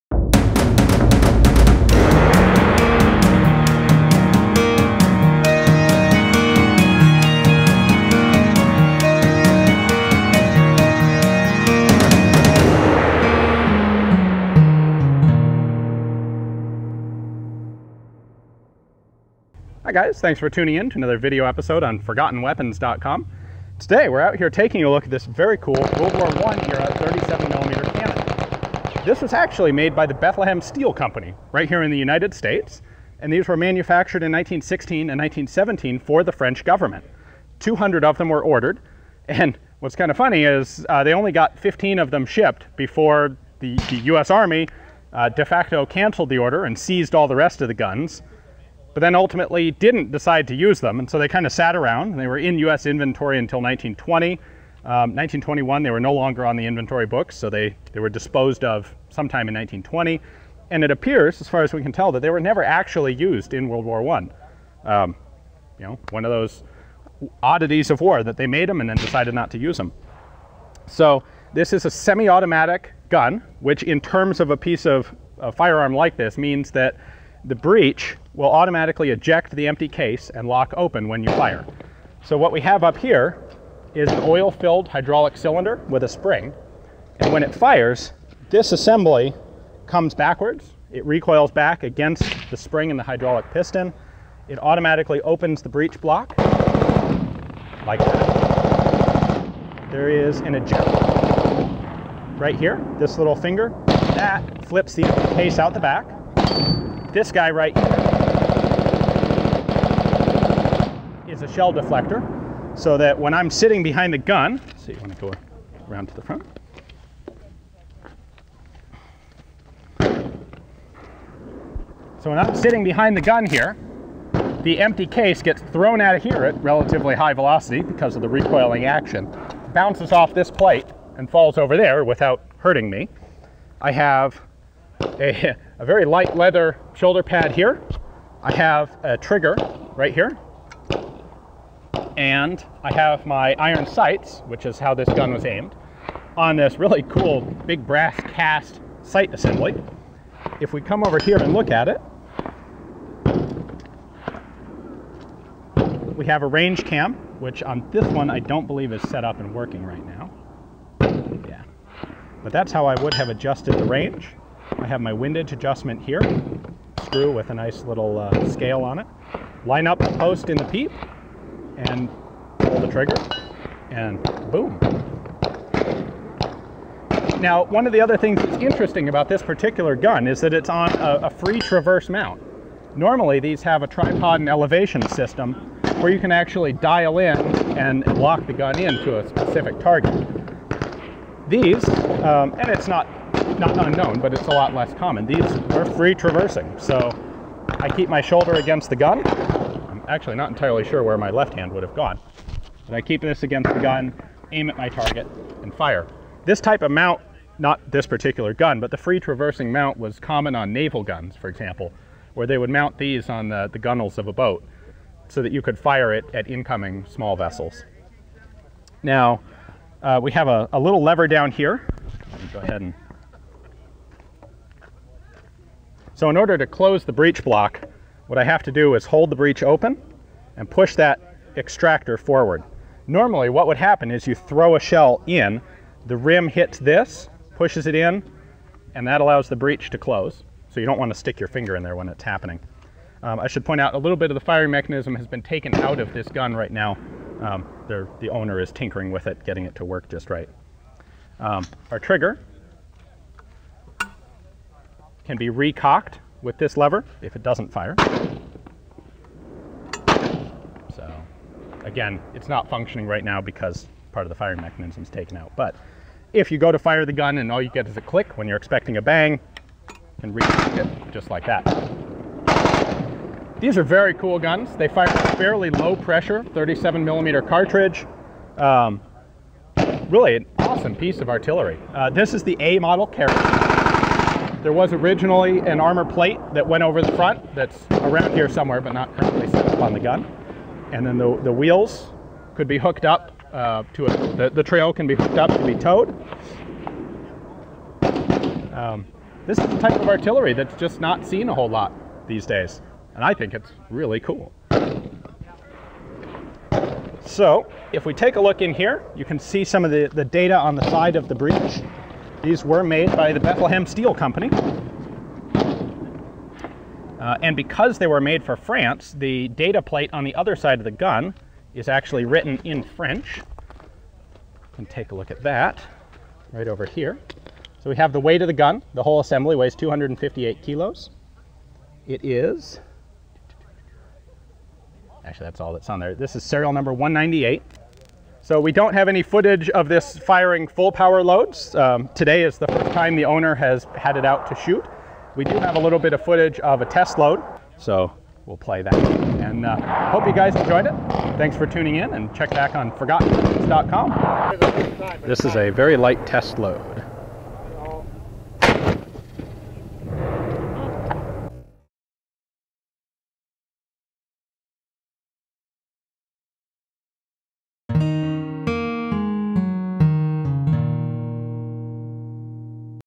Bethlehem Steel 37mm Cannon - WWI Era
The 37mm gun was found in many guises during World War One - that caliber was the smallest allowed to use exploding projectiles by the 1899 Hague accords. Every nation in the world, it seems, used 37mm guns of one type or another. Well, one particular version I had the chance to look at was built by Bethlehem Steel in Pennsylvania for the French Army. Chambered for the same 37x136mm Hotchkiss Heavy cartridge used in US naval service, 200 were purchased in 1916. Only 15 were actually shipped before the US Army seized the bulk of the order in 1917 - but they were never put into any sort of service by the US military. The French tested the guns for suitability as n infantry gun, anti-tank gun, anti-aircraft gun, and naval landing gun - and found unsuitable for all roles. It was probably tested only as a backup in case the redesign of the Mle 1916 Tir Rapid ran into problems.

At any rate, the 15 guns sent to France were sent back at the end of the war, and the guns remained in US Army inventory until 1921, when they appear to have been distributed out the National Guard units.

I think this is a very cool gun for the present-day enthusiast. In addition to the historical links to WWI, it has the practical benefits of being relatively light and using ammunition relatively inexpensive to reload. And, of course, the free mount and shoulder rest and iron sights give it more the feel of a shoulder rifle than later light artillery that use precisely adjusted mounts. This one is just more fun to shoot! Alas, there are very few still in existence. Perhaps an opportunity for someone who wants to make a reproduction Big Boy's Toy?